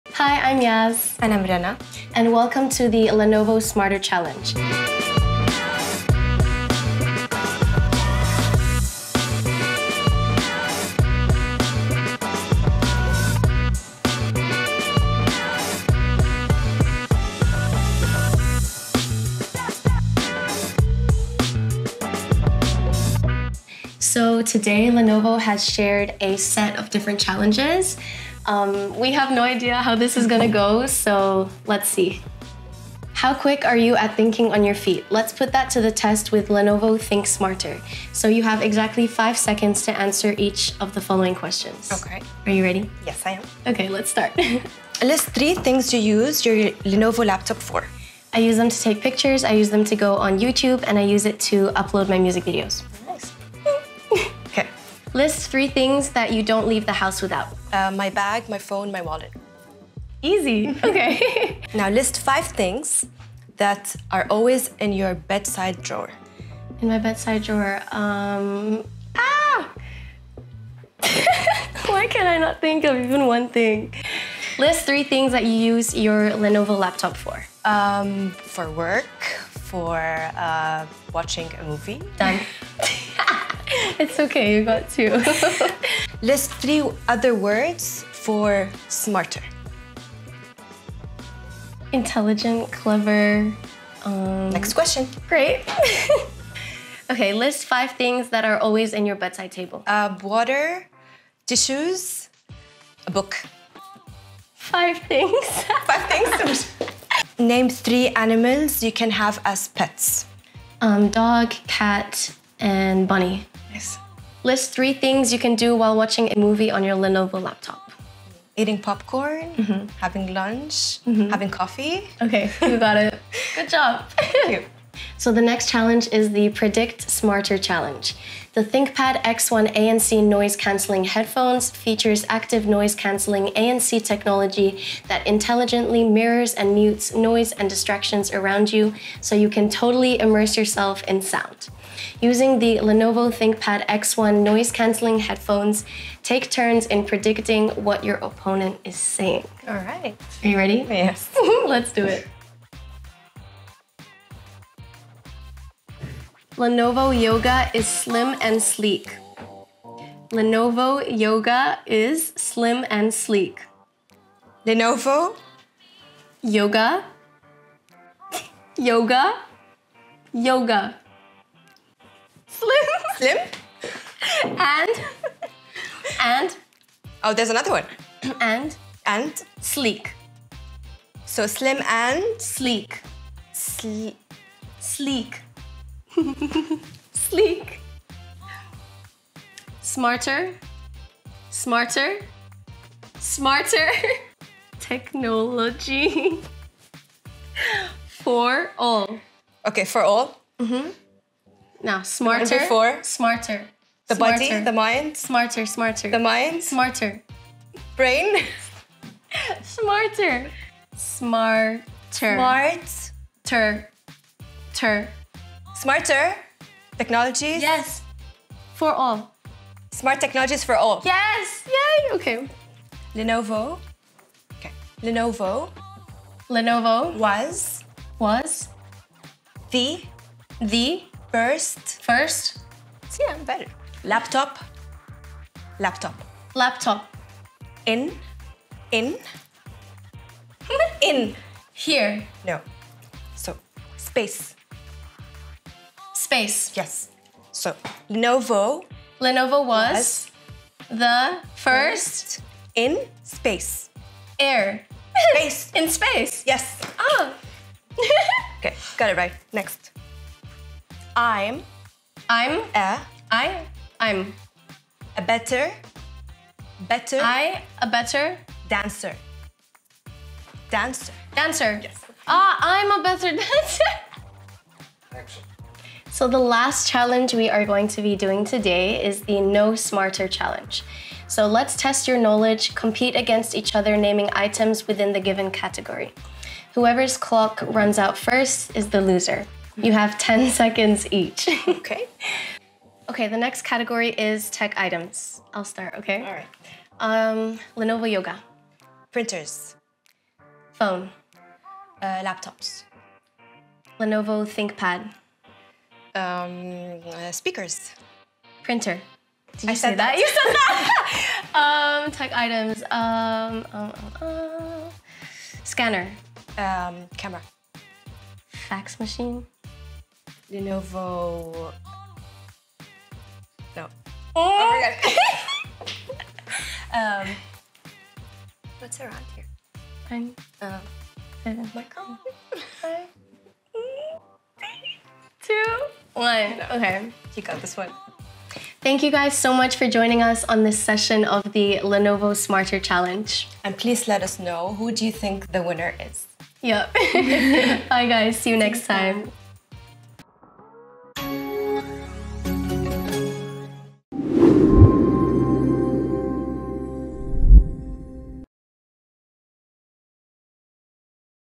SmarterChallenge with Yas Vocals
Who’s the quickest at thinking on the go?
Let’s find out with Yas Vocals and Rana, in this session of Lenovo

Intel® Core™ i7⁣ processor ⁣